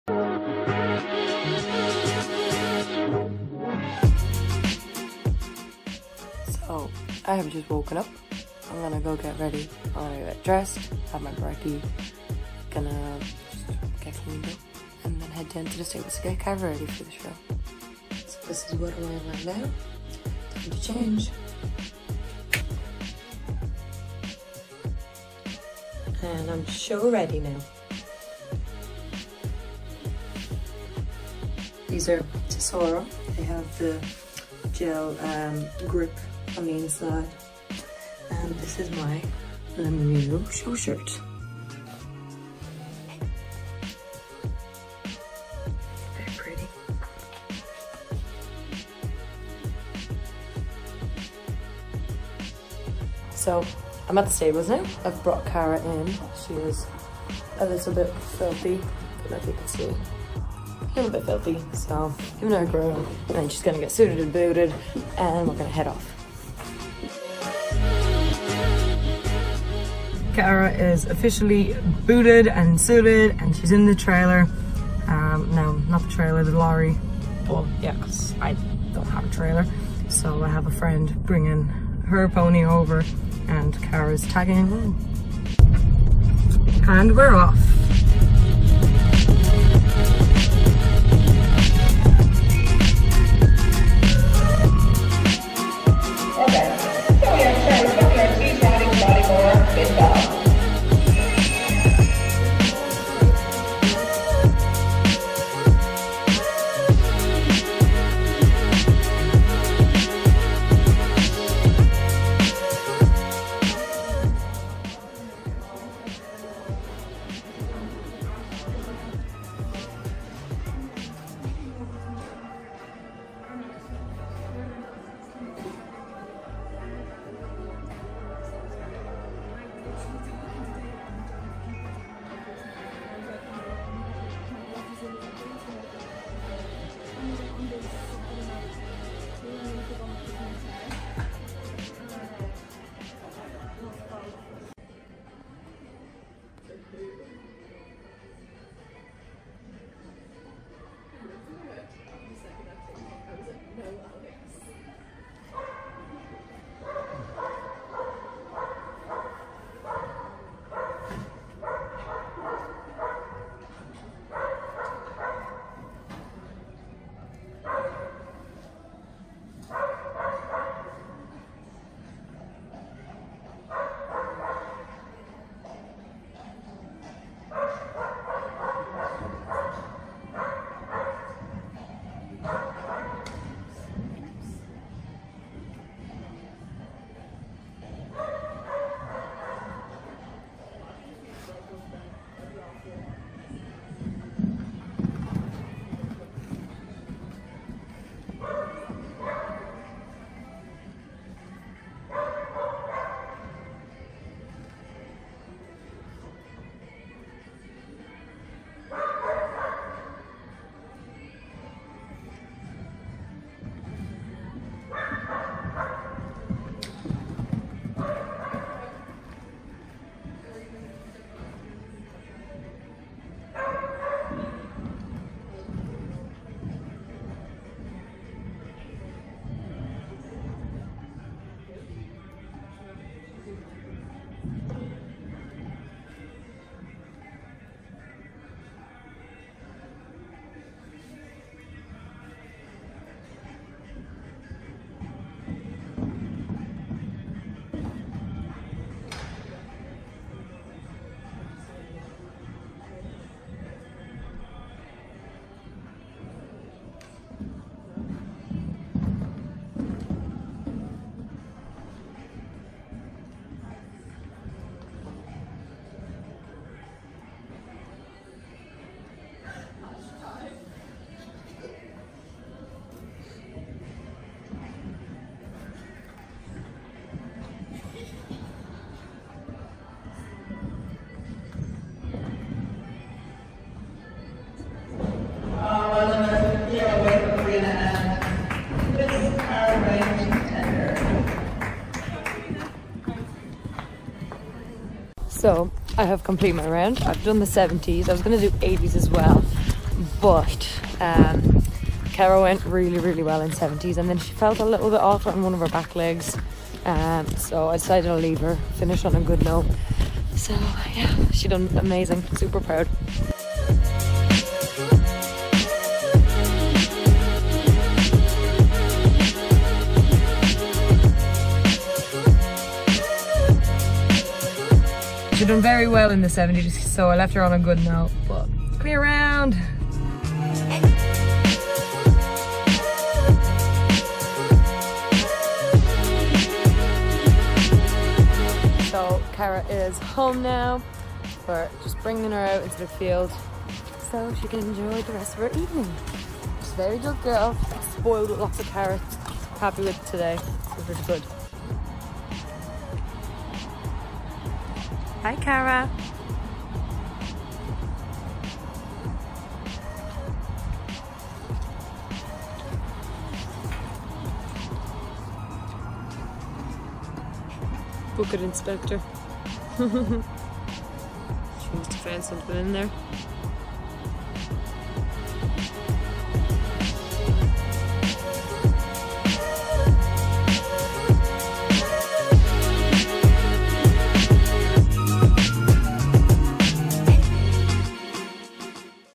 Jag Equestrian Show Vlog, Cara's first-ever indoor show
This is Cara's first-ever show in an indoor arena. We jumped 70s and were registered to jump in 80s but Cara felt a bit off towards the end of her 70s round and plus she had done an amazing job in the 70s so i decided to leave her on a good note.
This is also my first ever show vlog and yes i know its cringy XD I was too shy to record myself and talk while people were around so i know i look a bit awkward.